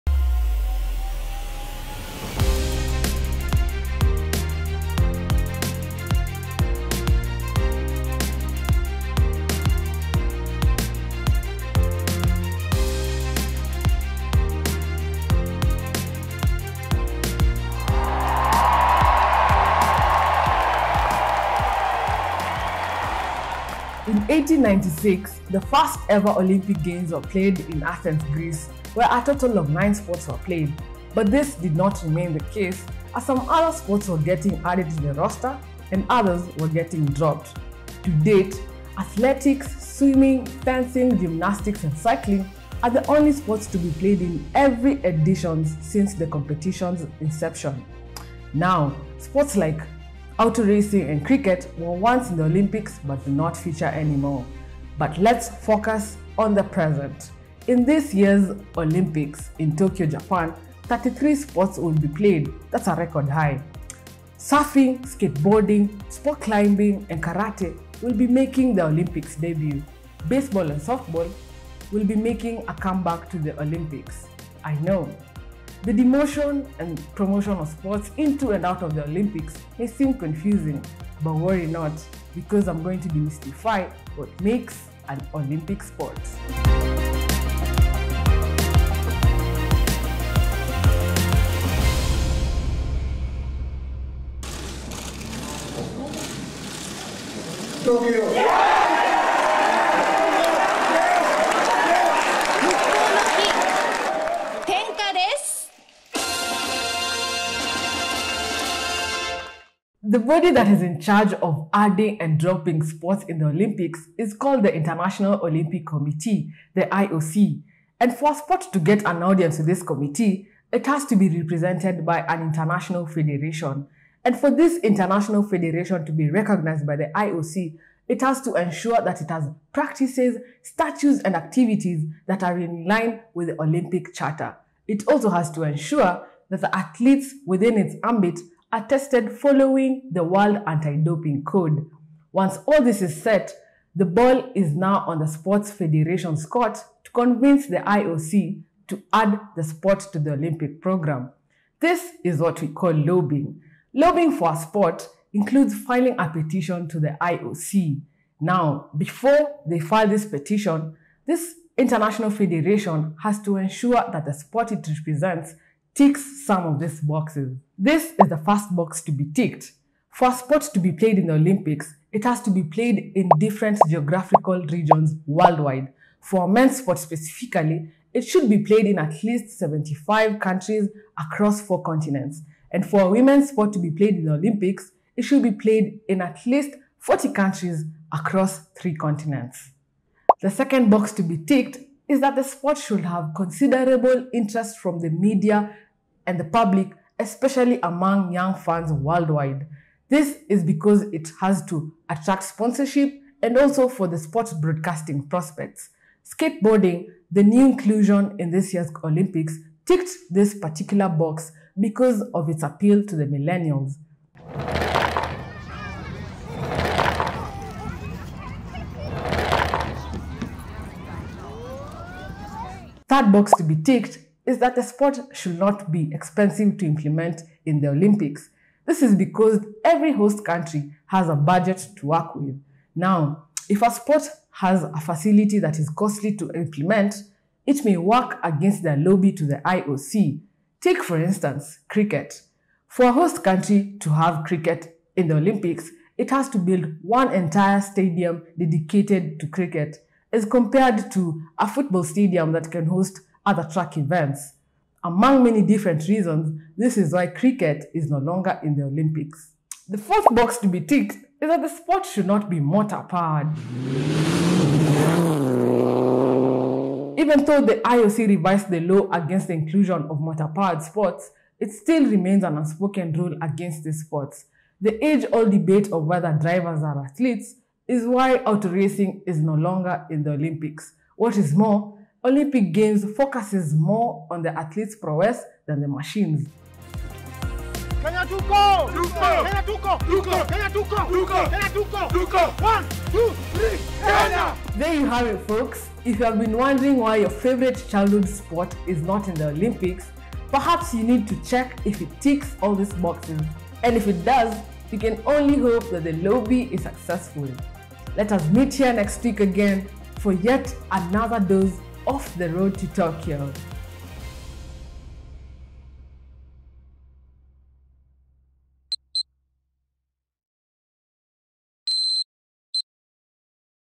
What Makes An Olympic Sport?
In 1896, the first-ever Olympic Games were held in Athens, Greece, where a total of 9 sports were featured. But the Olympics didn't stay static. Over the years, the number of sports played at the Olympics fluctuates, with some games gaining a spot on the roster while others getting dropped.

To date, Athletics, Swimming, Fencing, Gymnastics and Cycling are the only five sports to have featured in all Olympic Games since the competition's inception. Sports such as Cricket and Auto Racing were once featured but have since been relegated.

In this year's forthcoming Olympic Games in Tokyo, Japan, a total of 33 games will feature including surfing, skateboarding, sport climbing and karate, all of which are making their Olympics debut. Baseball and softball will be making a comeback.